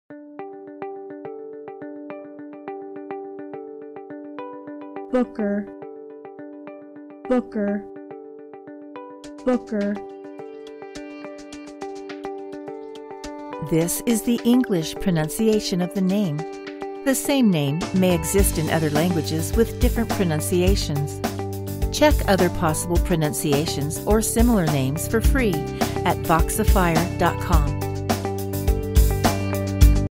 English Pronunciation of Booker - Voxifier.com
This is the English pronunciation, gender, and phonetics recording of the name Booker. The same name may exist in other languages with different pronunciations.